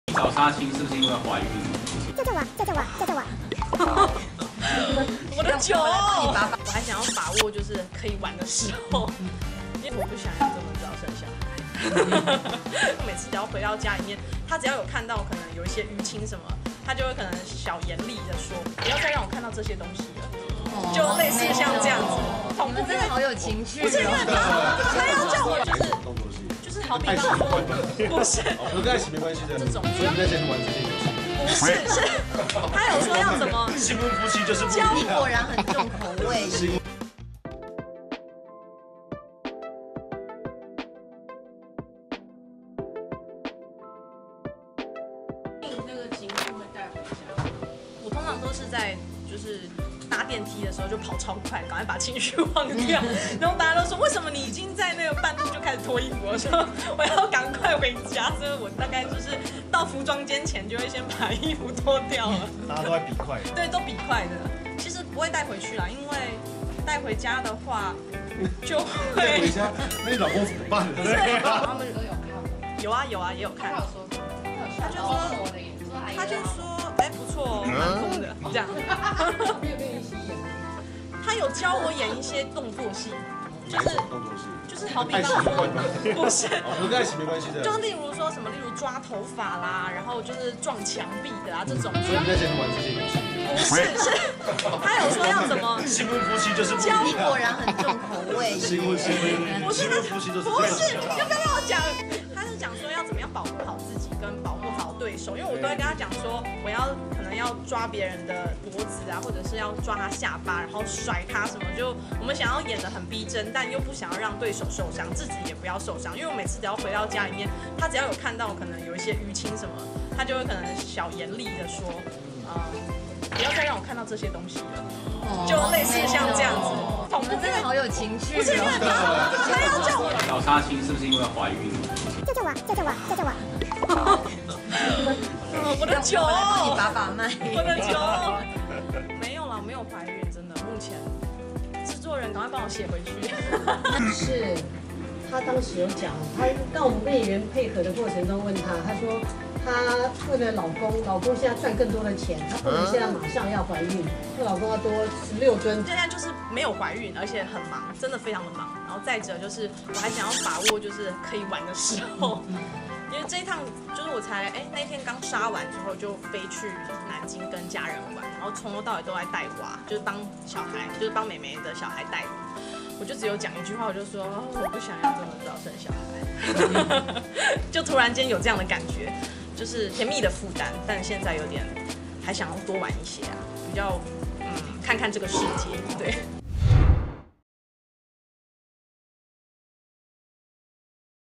Energy書偉新婚7個月罕動怒！ 謝京穎吐實情遭虧：好有情趣｜TVBS娛樂頭條
女星謝京穎和「Energy」團員張書偉6月舉辦婚禮，婚後馬上投入民視八點檔《愛的榮耀》拍攝，卻因在螢光幕前臉型有些水腫，讓外界懷疑有孕，她10日出席記者會鬆口肚皮動靜，謝京穎在劇中飾演反派「林安琪」，有許多衝突戲碼，手腳都因此有些瘀青，讓老公張書偉相當又心疼又氣憤，警告她：「不要再讓我看到這種東西（瘀青）了！」讓一旁的吳婉君笑說：「你們好有情趣喔～。」還在家示範動作戲，教導謝京穎怎樣抓頭、撞牆不會受傷，也會幫她擦藥，語重心長說：「重點是不要受傷！」。